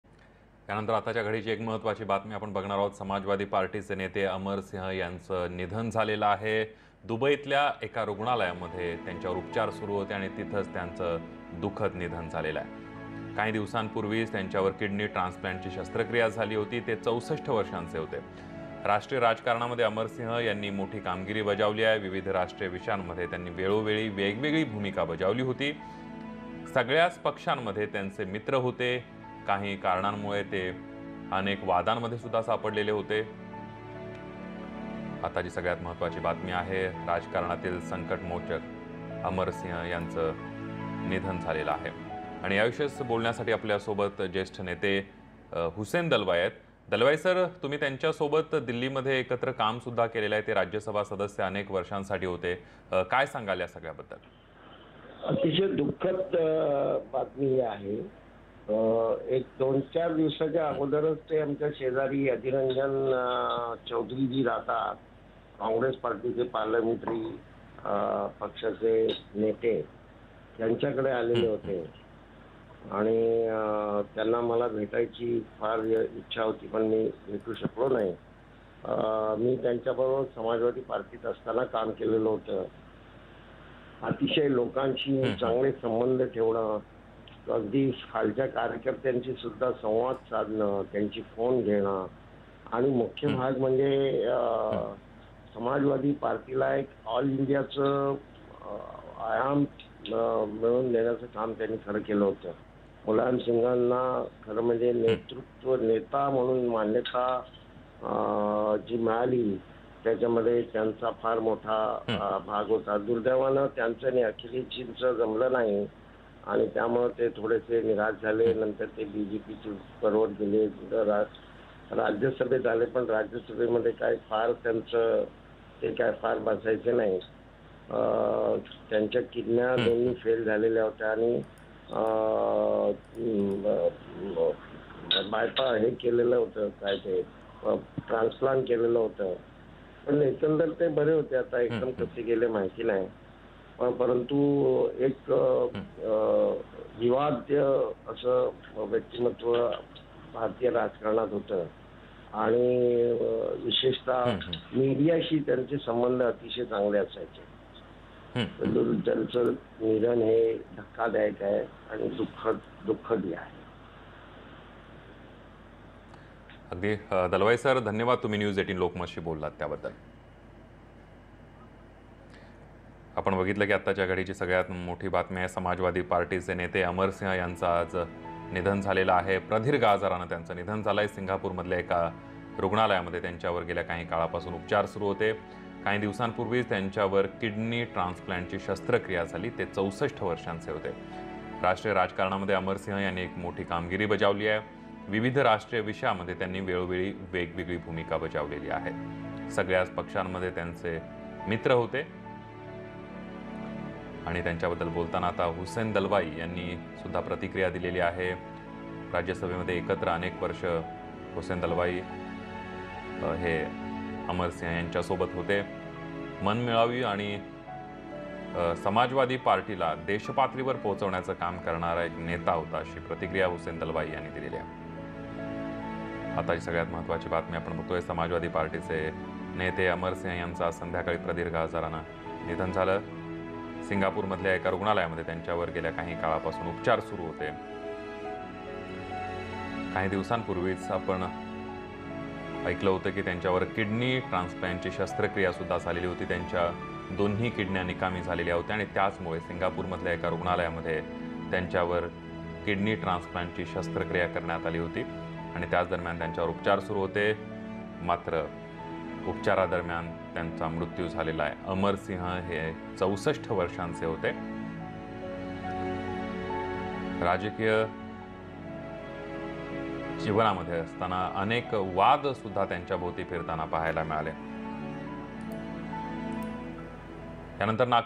जेष्ठ नेते Amar Singh यांचे निधन
जेष्ठ नेते अमर सिंग यांचे निधन झाले आहे. दुबईतील एका रुग्णालयामध्ये त्यांचं निधन झालं आहे. राज्यसभेचे खासदार अमर सिंग यांचं निधन झालं आहे.

/News18Lokmat